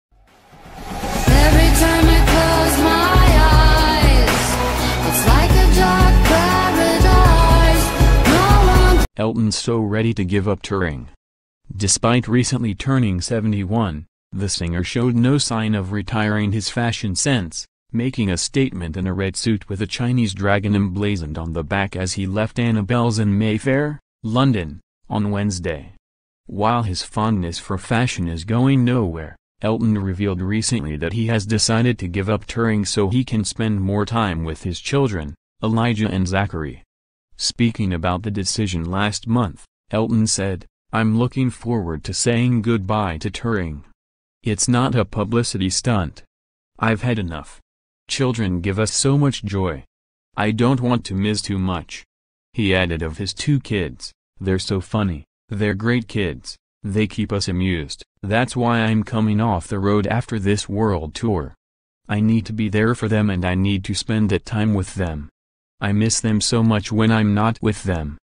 Elton's so reddy to give up touring
Elton's so reddy to give up touring. Despite recently turning 71, the singer showed no sign of retiring his fashion sense, making a statement in a red suit with a Chinese dragon emblazoned on the back as he left Annabel’s in Mayfair, London, on Wednesday.

While his fondness for fashion is going nowhere, Elton revealed recently that he has decided to give up touring so he can spend more time with his children, Elijah and Zachary.

Speaking about the decision last month, Elton said: